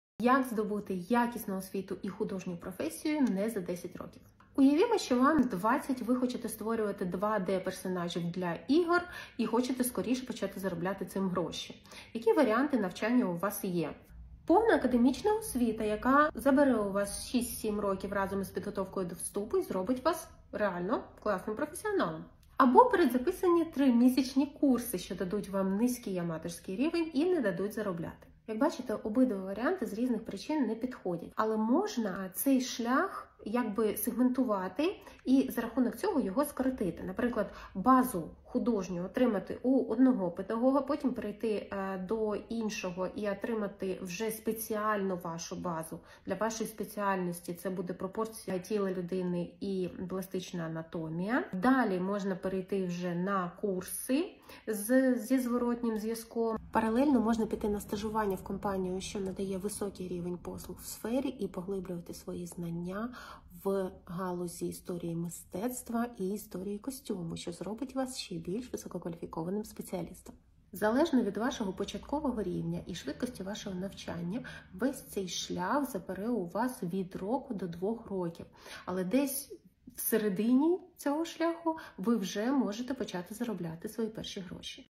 ЯК СПЛАНУВАТИ ХУДОЖНЮ ОСВІТУ В ДОРОСЛОМУ ВІЦІ? Курси чи вища освіта? Навчання на творчі професії
Цей узагальнений план є зразком більш детального індивідуального плану навчання, які я роблю. Останнім часом це моя пристрасть і я планую займатися ще й продюсуванням молодих майстрів в майбутньому ⭐️

Без плану ж навчання малюванню у дорослому віці більше схоже на подолання психологічної травми з безкінечним пошуком рішень і спеціалістів, витратою часу і грошей без явного результату.

Давайте краще планувати і швидко рухатись до мети 🌝